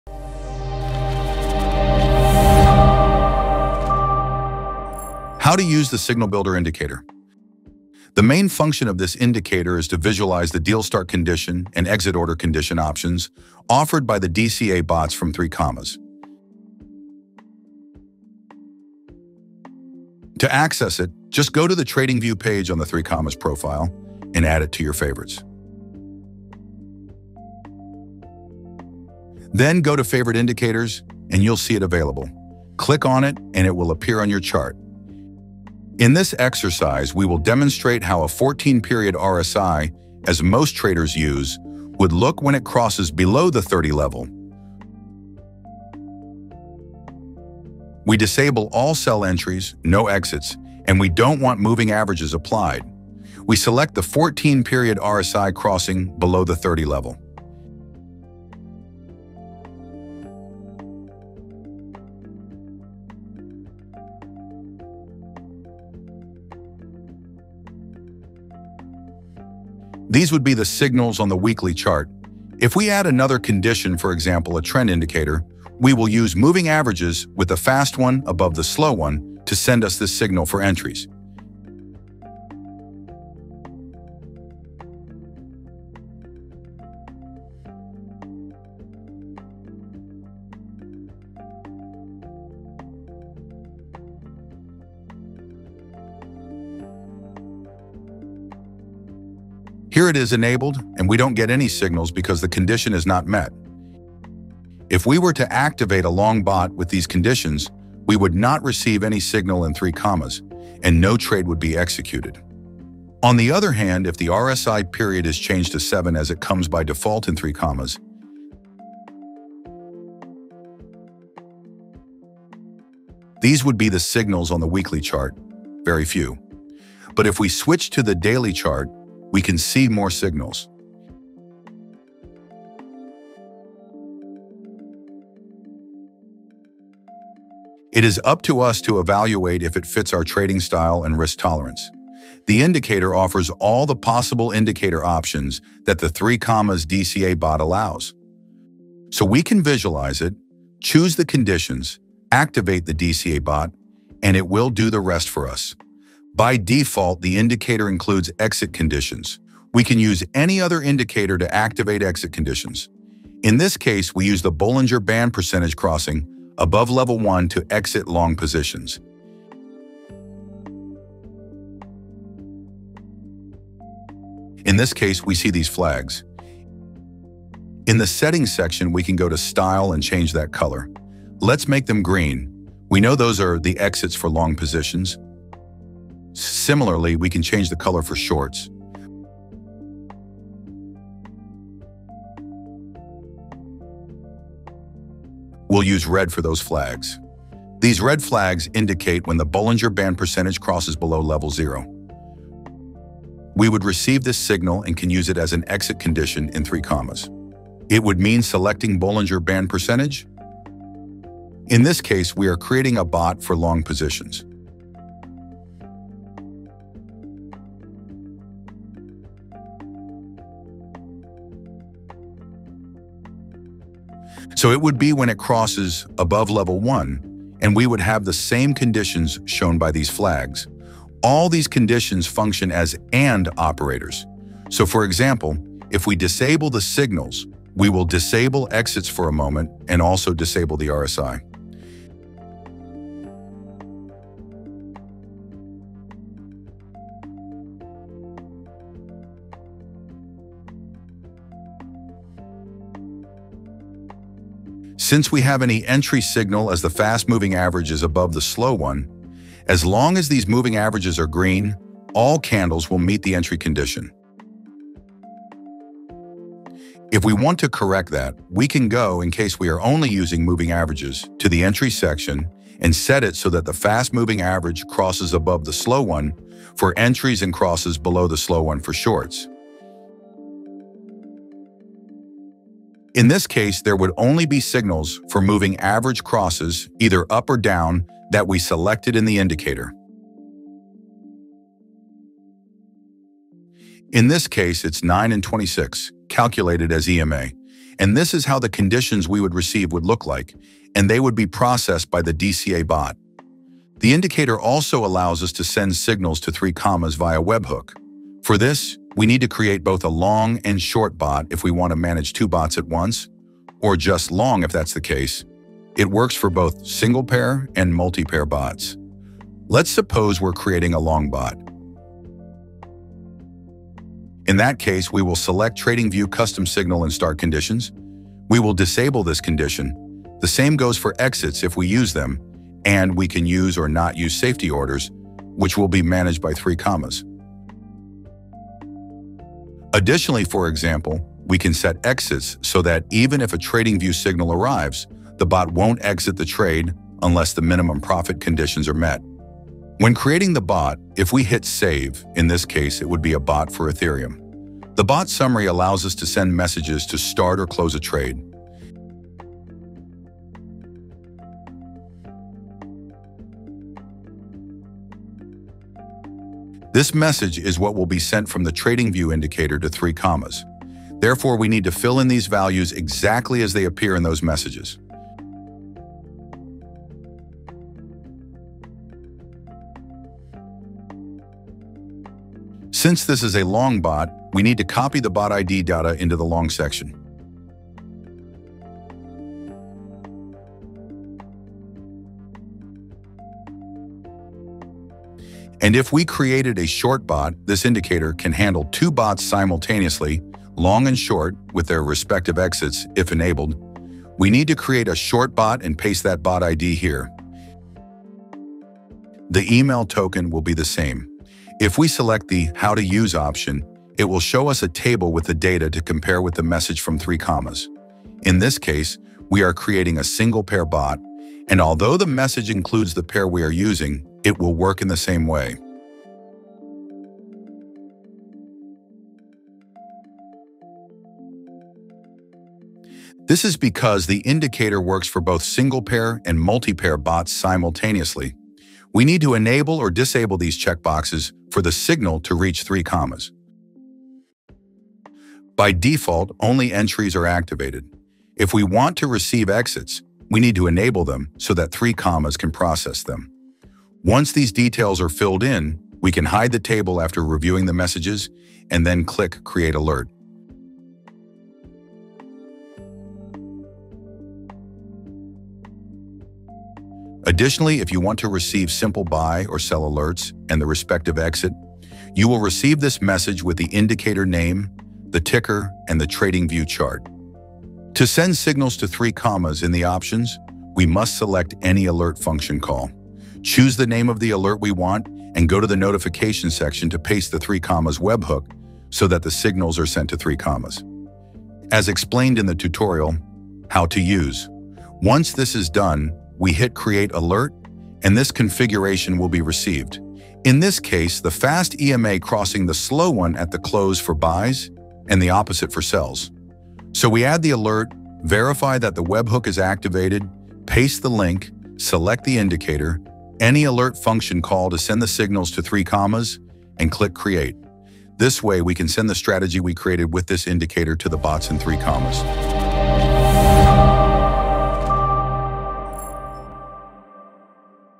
Master TradingView Signal Builder: Create Custom Buy/Sell Signals with 3Commas Bots
Unlock the full potential of 3Commas bots with our in-depth guide on using the TradingView Signal Builder Indicator! This tutorial walks you through customizing DCA bot signals to elevate your trading strategy in the crypto market.